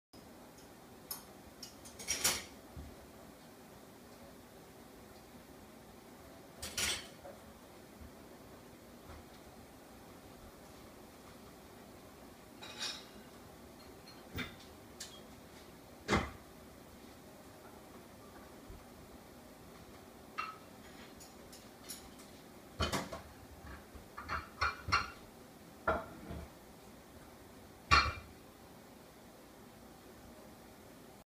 Pacific Banana Slug on a cabin window
The large upper tentacles detect light and movement, similar to eyes. The smaller, lower tentacles detect chemicals, similar to a nose. If a slug happens to lose a tentacle, it can regrow a new one.

The fleshy lips hide a tongue with small teeth that the slug uses like sandpaper to grind food.

(Kitchen sounds in the background)

Recorded: 2016 May 21 Sat 1847